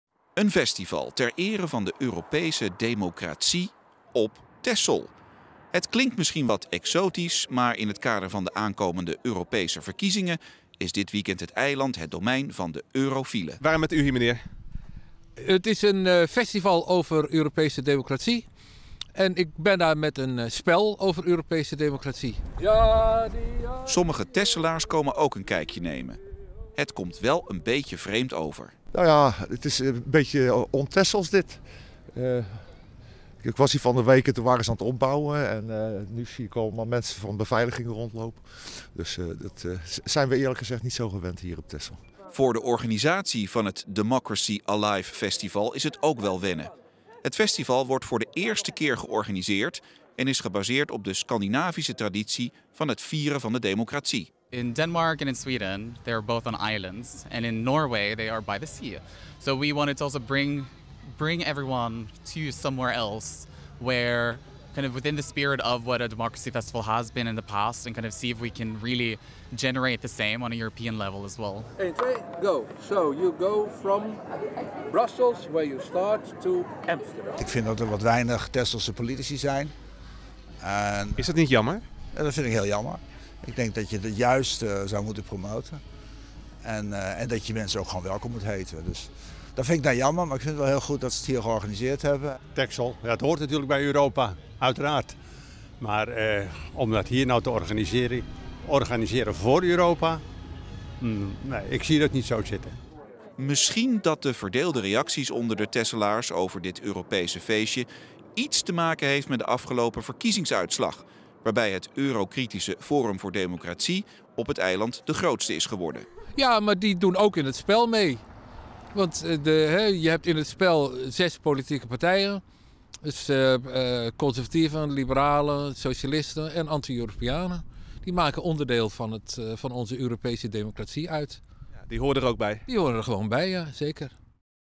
Festival voor viering Europese democratie op Texel: "Zie dat niet zo zitten"
Texel staat dit weekend in het teken van de Europese democratie tijdens het festival Democracy Alive. Het is voor het eerst dat het gratis festival, die in het kader van de Europese verkiezingen wordt georganiseerd.